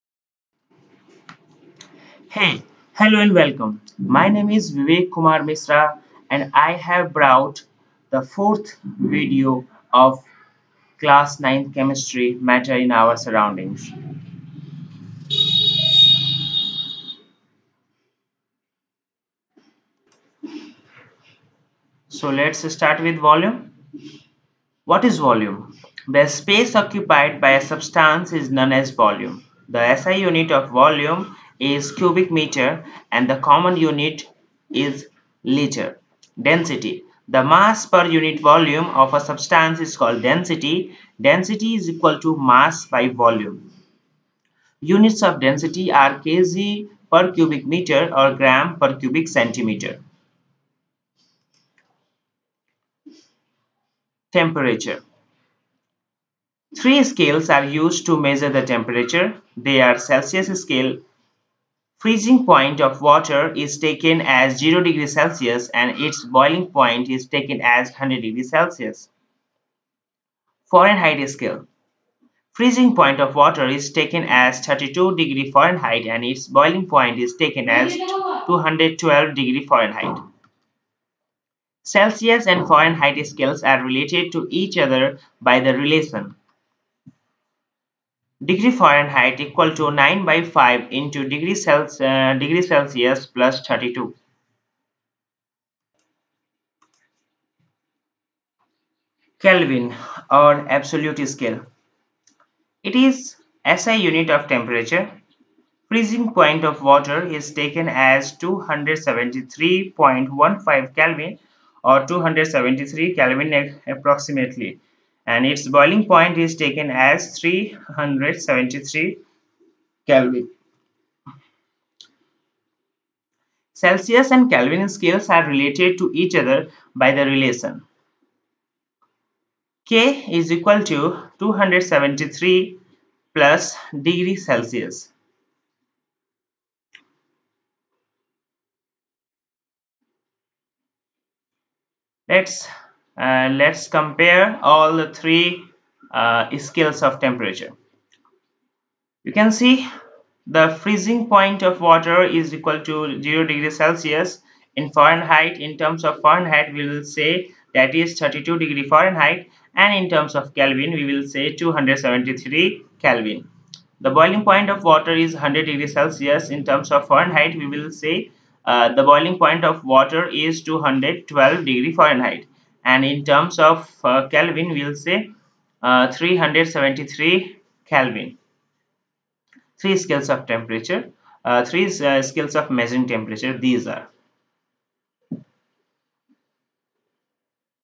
Class 9| Chemistry| Chapter 1| Matter In Our Surrounding| Part 4
In this video you will learn about density and volume along with the scales of measuring the temperature.